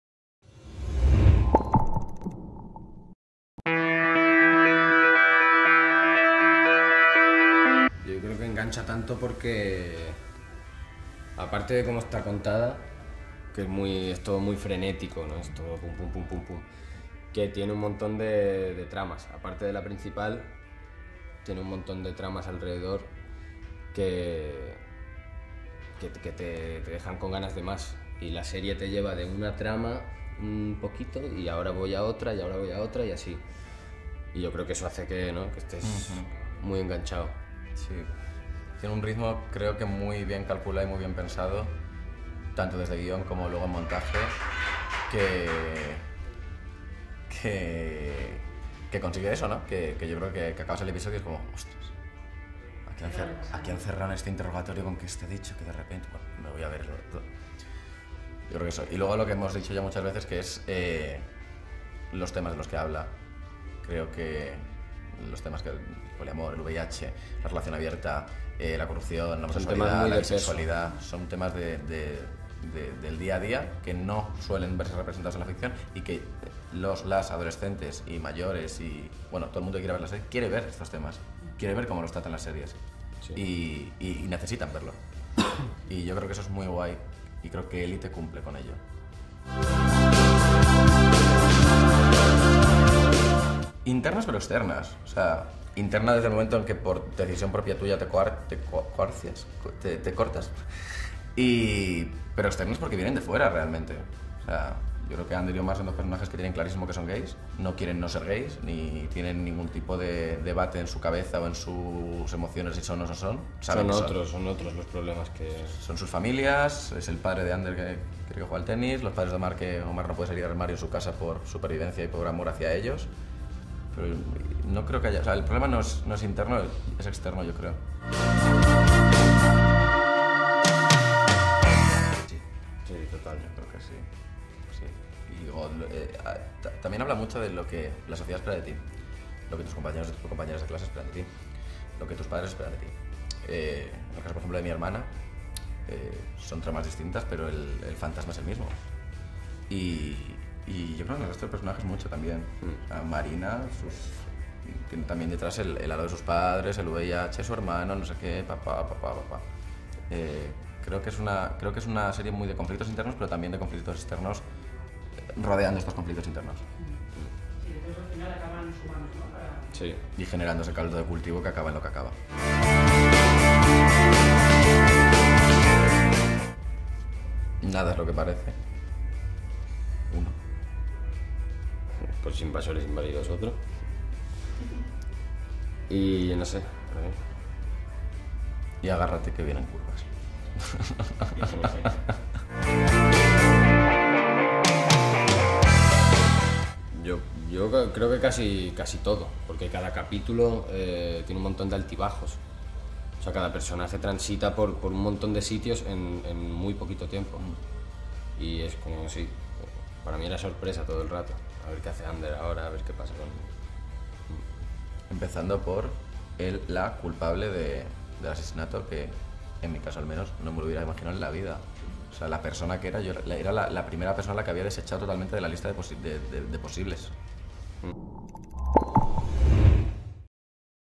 'Élite': Entrevista a Omar Ayuso (Omar) y Arón Piper (Ander)
Con motivo del estreno de 'Élite' entrevistamos a dos de sus protagonistas: Omar Ayuso (Omar) y Arón Piper (Ander).